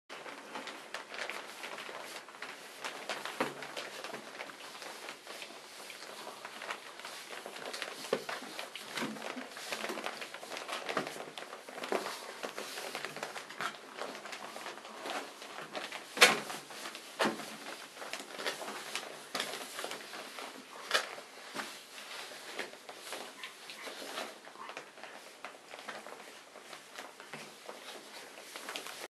Cachorros con 20 dias Jugando Pastor Belga Groenendael nacidos el 11 enero 2010
Los cachorros con 20 dias ya estan jugando entre ellos, se muerden y gruñen.